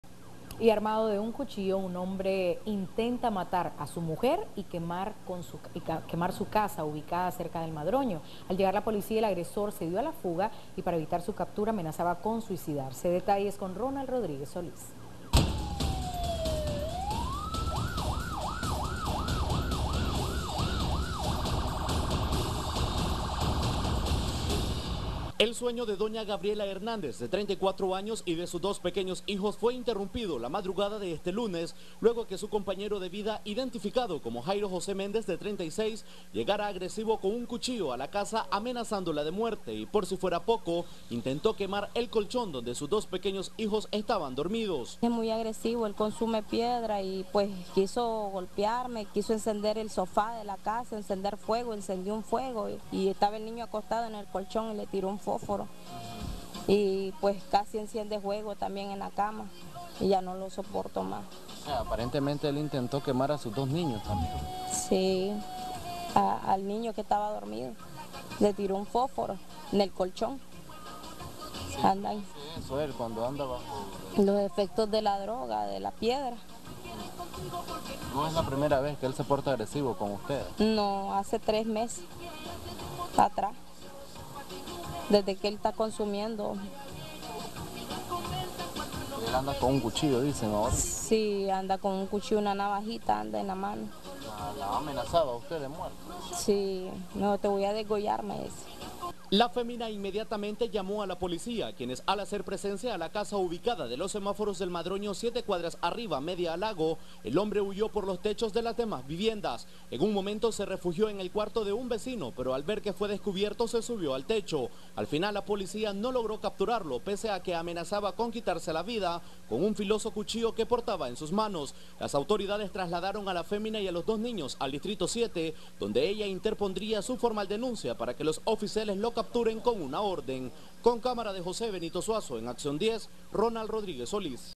Armado con un cuchillo un hombre intenta matar a su mujer
Para evitar su captura amenazaba con suicidarse. El hombre pretendía asesinar a su mujer y luego incendiar su casa, al ser avisada la policía el hombre huyó y amenazó con quitarse la vida para evitar su captura